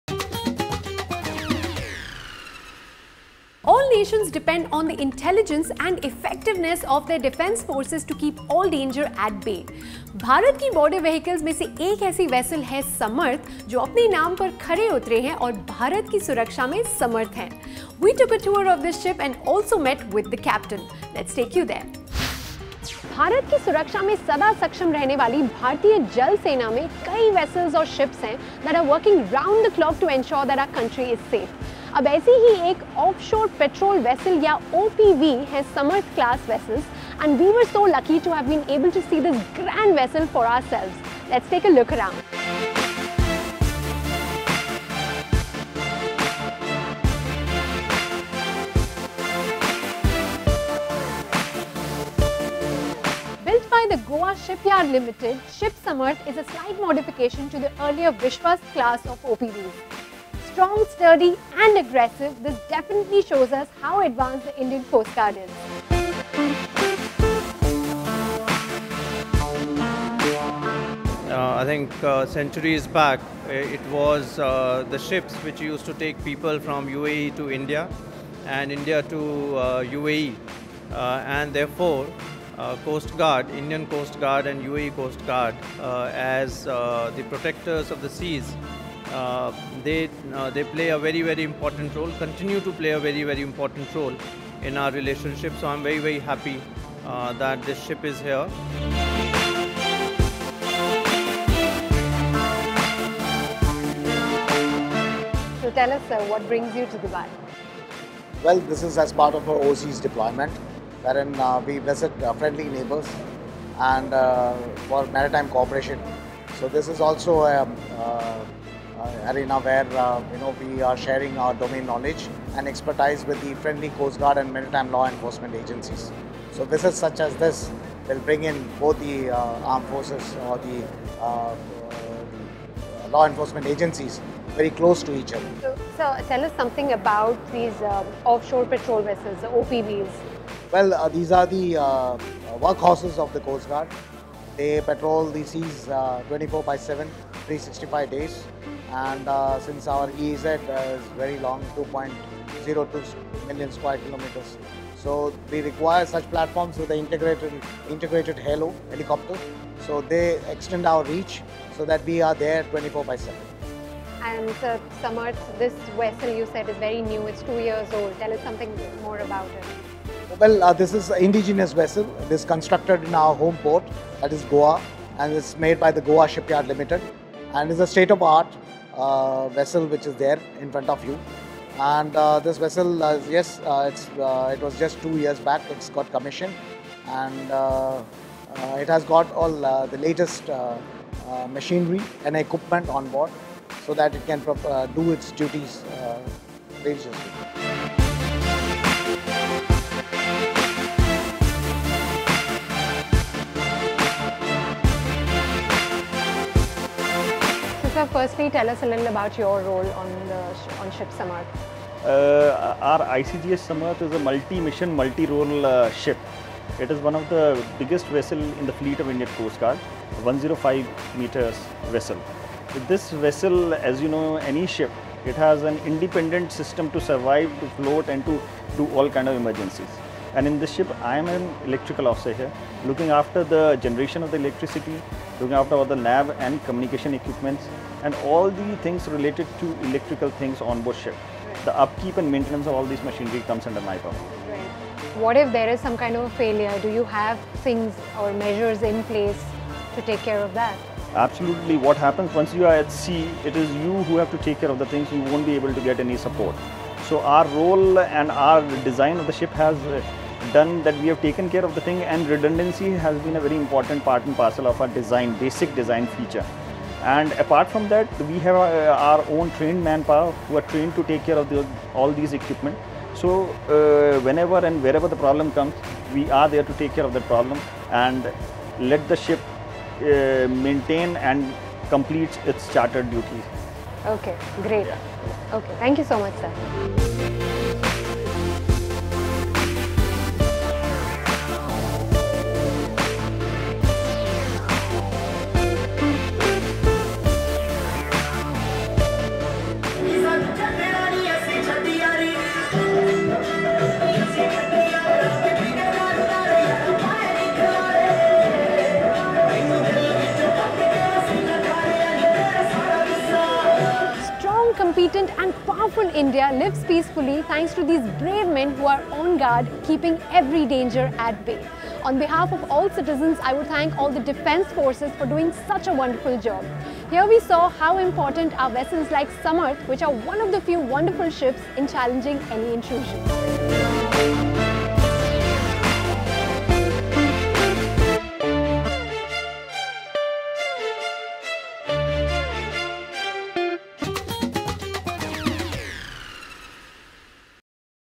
ICGS Samarth | Goodwill Visit to UAE
We have a gem from the Indian Defense vehicle called Samarth that is a patrol ship contributing hugely towards the safety of India.